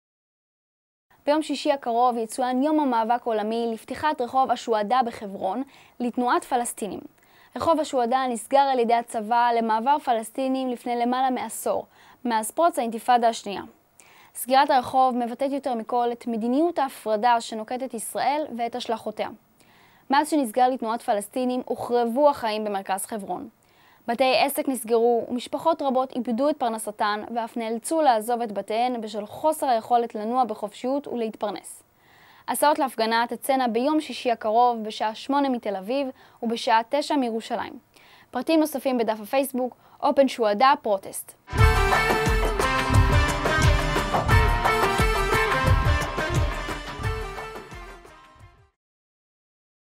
חוזרים לרחוב א-שוהדא
ביום שישי הקרוב (21/2/2014), יצוין יום המאבק העולמי לפתיחת רחוב א-שוהדא בחברון לתנועת פלסטינים. רחוב א-שוהדא נסגר על ידי הצבא למעבר פלסטינים לפני למעלה מעשור, מאז פרוץ האינתיפאדה השנייה. סגירת הרחוב מבטאת יותר מכל את מדיניות ההפרדה שנוקטת ישראל ואת השלכותיה. מאז שנסגר לתנועת פלסטינים, הוחרבו החיים במרכז חברון - בתי עסק נסגרו, ומשפחות רבות איבדו את פרנסתן ואף נאלצו לעזוב את בתיהן בשל חוסר היכולת לנוע בחופשיות ולהתפרנס.